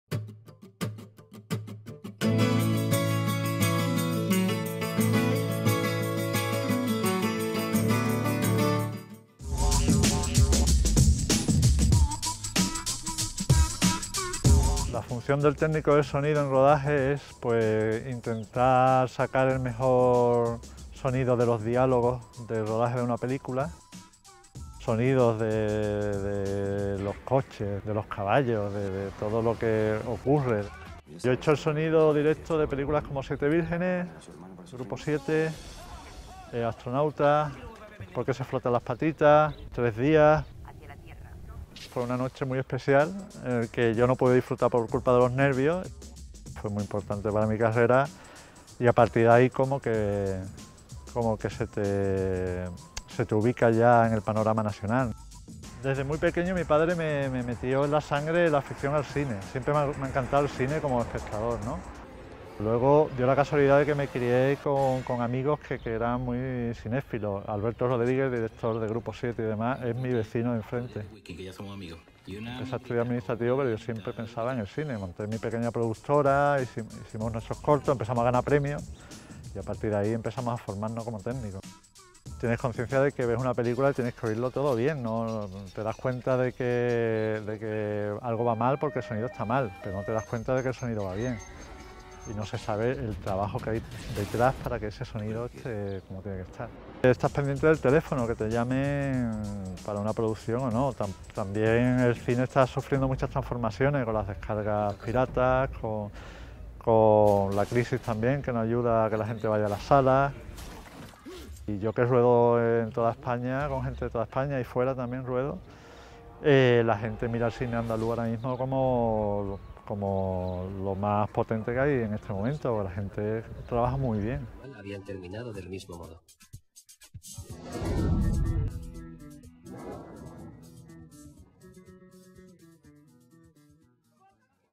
HDL Daniel de Zayas, un técnico de sonido en lo más alto del cine español
El sevillano Daniel de Zayas está considerado uno de los mejores técnicos de sonido directo del cine español. Ha trabajado en películas como 'Siete Vírgenes', 'Grupo7', 'Astronautas', 'Por qué se frotan las patitas' y 'Tres días'. Fue su trabajo en esta última producción el que le llevó a ganar el Goya en 2009. Este aficionado al cine desde muy corta edad destaca la potencia del cine andaluz en el actual panorama nacional e internacional.